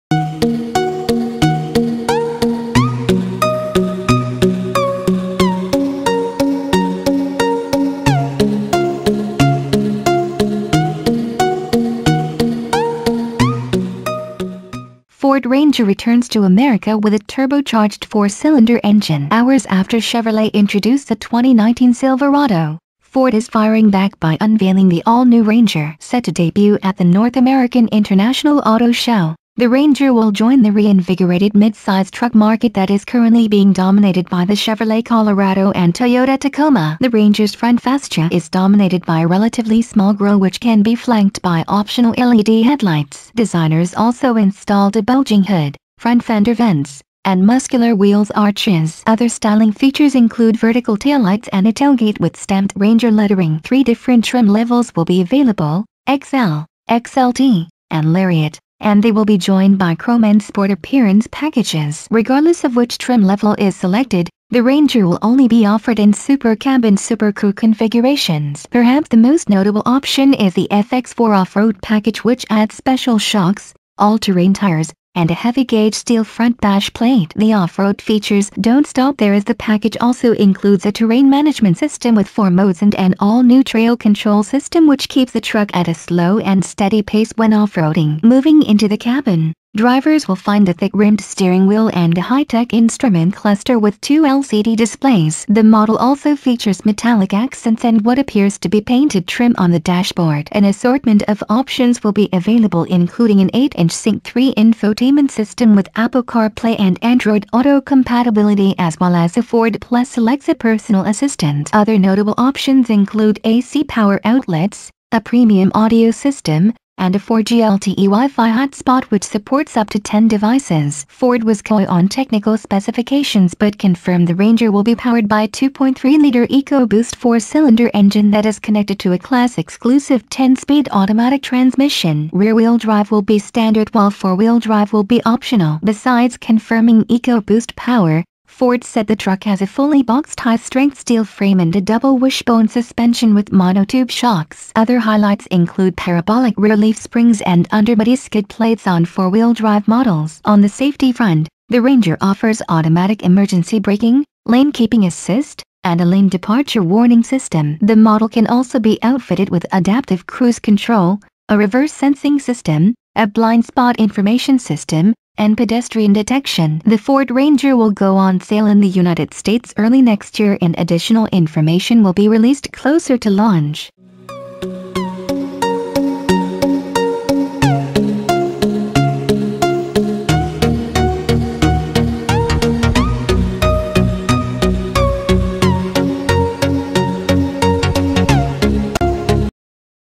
Ford Ranger Returns To America With A Turbocharged Four Cylinder Engine
Hours after Chevrolet introduced the 2019 Silverado, Ford is firing back by unveiling the all-new Ranger.

Set to debut at the North American International Auto Show, the Ranger will join the reinvigorated mid-size truck market that is currently being dominated by the Chevrolet Colorado and Toyota Tacoma.

The Ranger's front fascia is dominated by a relatively small grille which can be flanked by optional LED headlights. Designers also installed a bulging hood, front fender vents, and muscular wheels arches. Other styling features include vertical taillights and a tailgate with stamped "RANGER" lettering.